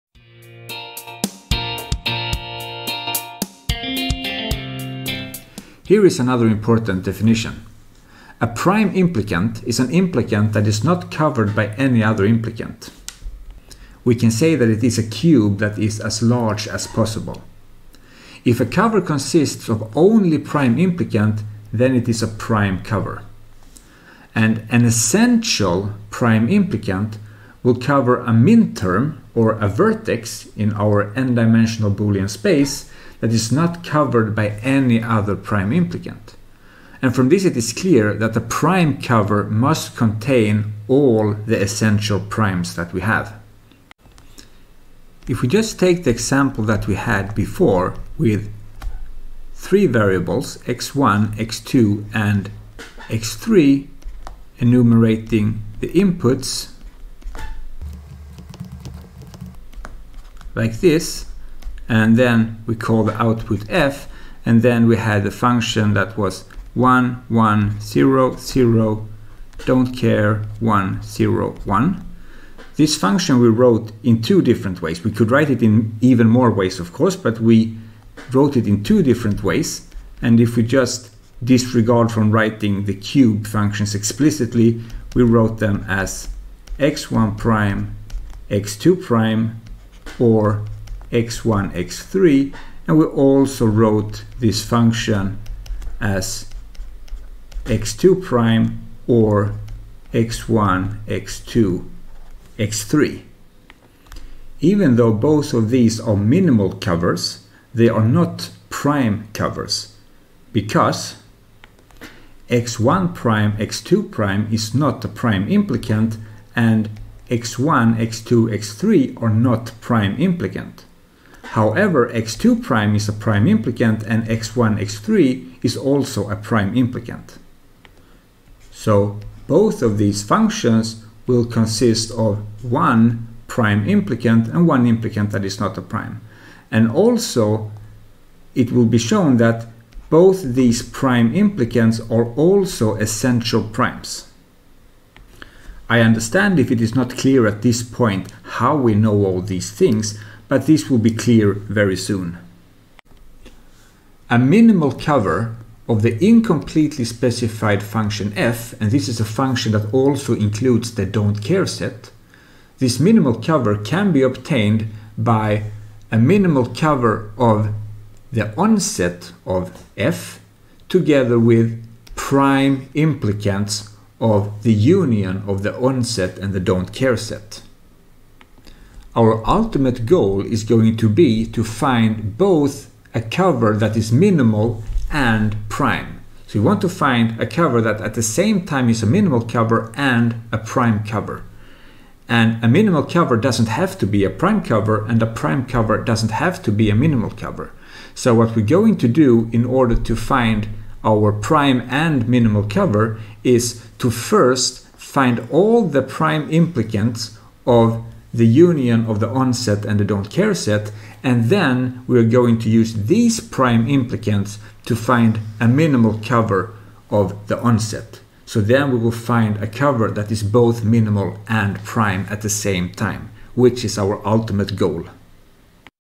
7.2 Prime Implicants and Essential Primes
We define prime implicants and essential prime implicants. Prime implicants are the ones we are looking for because they help us get the most compact way of describing a function, i.e., to get the minimal cover.